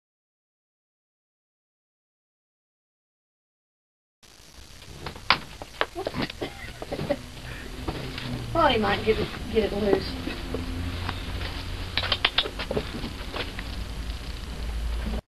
New pup 10.09
Our latest rescue is a Catahoula mix about 4 months old.